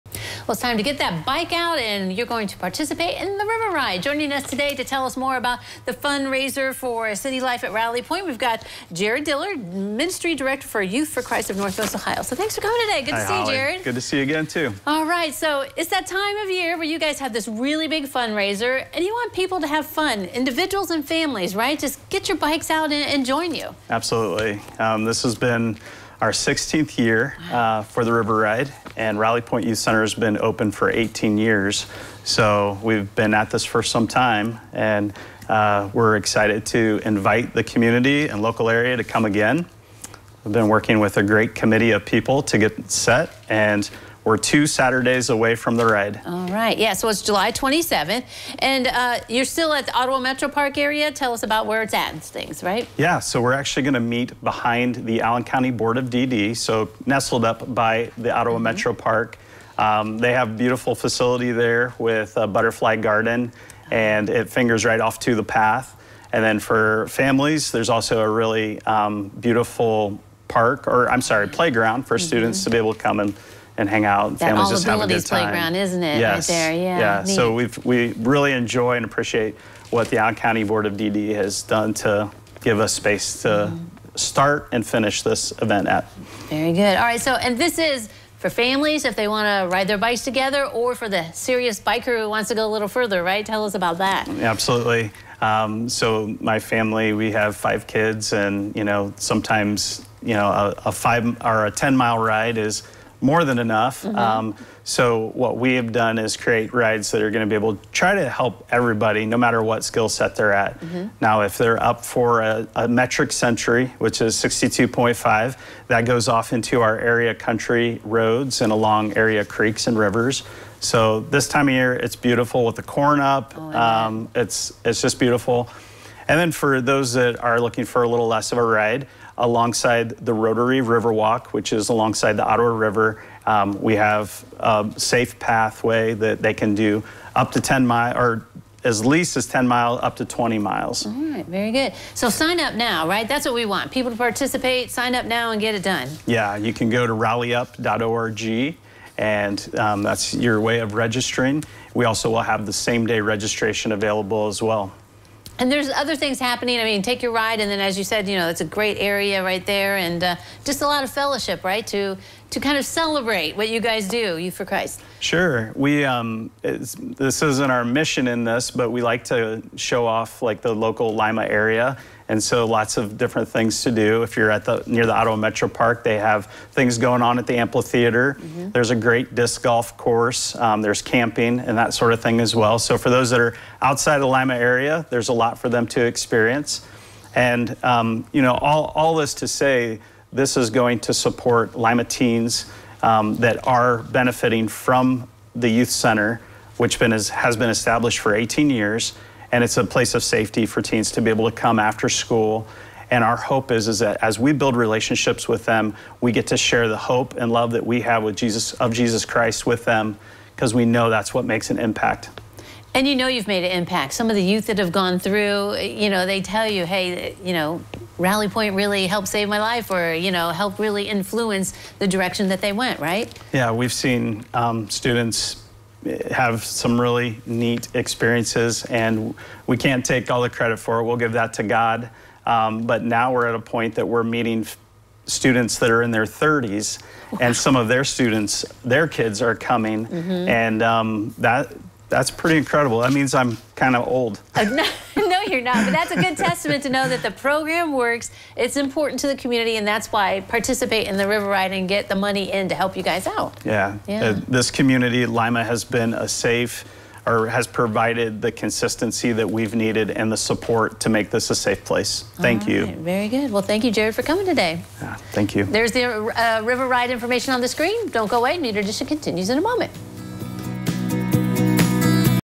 2024 River Ride for Rally Point
The annual River Ride is coming up to help raise money for Rally Point, a center that helps area youth!